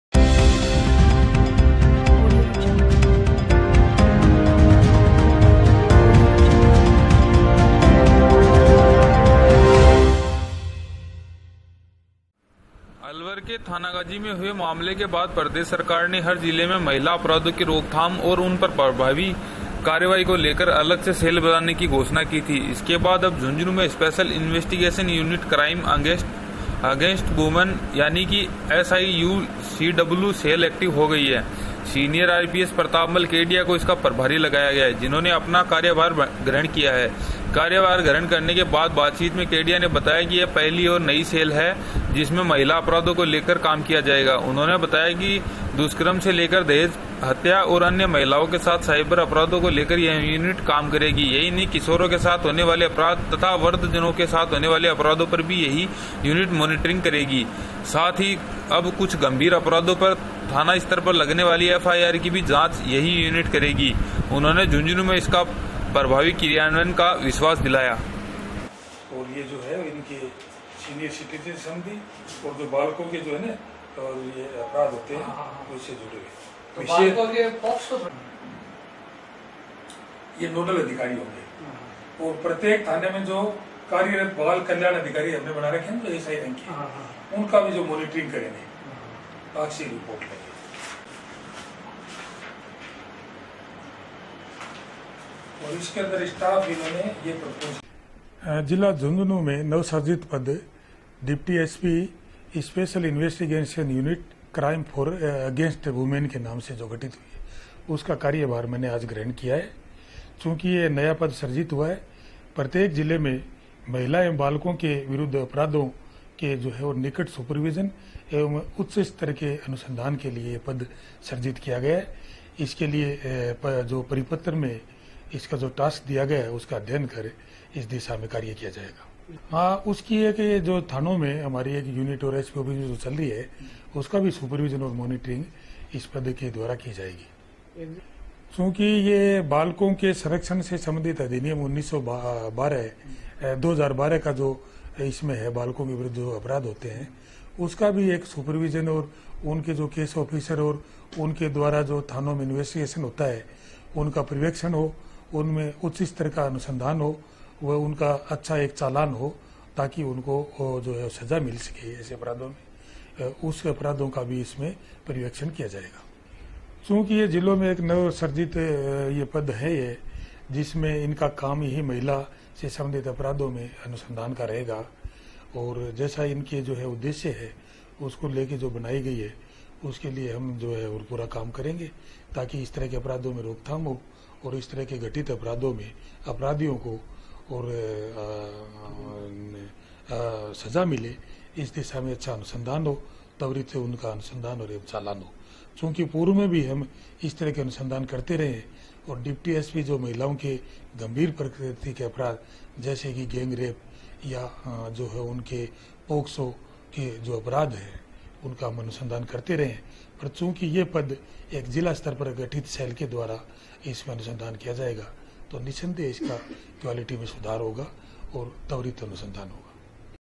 JHUNJHUNU I झुंझुनूं में अब महिला अपराधों पर कसी जाएगी लगाम
लवर के थानागाजी में हुए मामले के बाद प्रदेश सरकार ने हर जिले में महिला अपराधों की रोकथाम और उन पर प्रभावी कार्रवाई को लेकर अलग से सैल बनाने की घोषणा की थी। इसके बाद अब झुंझुनूं में स्पेशल इंवेस्टीगेशन यूनिट क्राइम अंगेस्ट वुमन यानि कि एसआईयूसीडब्लू सैल एक्टिव हो गई है। सीनियर आरपीएस प्रतापमल केडिया को इसका प्रभारी लगाया गया है। जिन्होंने अपना कार्यभार ग्रहण किया है। कार्यभार ग्रहण करने के बाद बातचीत में केडिया ने बताया कि यह पहली और नई सैल है। जिसमें महिला अपराधों को लेकर काम किया जाएगा। उन्होंने बताया कि दुष्कर्म से लेकर दहेज हत्या और अन्य महिलाओं के साथ साइबर अपराधों को लेकर यह यूनिट काम करेगी। यही नहीं किशोरों के साथ होने वाले अपराध तथा वृद्धजनों के साथ होने वाले अपराधों पर भी यही यूनिट मॉनेटरिंग करेगी। साथ ही अब कुछ गंभीर अपराधों पर थाना स्तर पर लगने वाली एफआर की भी जांच यही यूनिट करेगी। उन्होंने झुंझुनूं में इसका प्रभावी क्रियान्वयन का विश्वास दिलाया है।